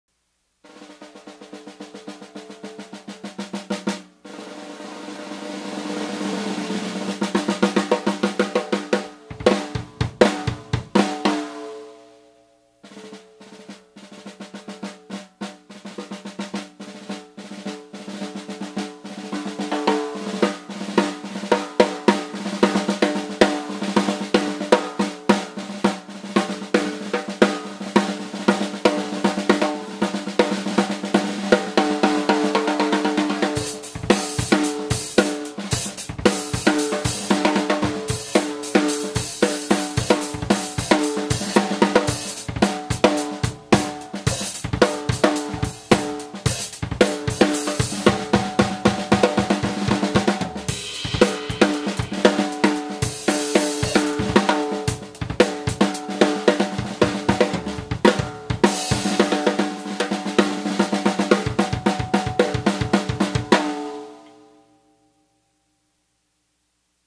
REMO (レモ) 114SE スネアサイド エンペラー ドラムヘッド
REMOのスネアサイドの中では厚いタイプのスネアサイドです。
打面ヘッドはREMO 114BA
他のスネアサイドと比較しやすいようにチューニングを揃えています。
ドメインを shirai-drums.com から に変更しています。随時作業しておりますが、リンク切れの場合はドメインのご確認をお願い致します。大変ご迷惑おかけいたします。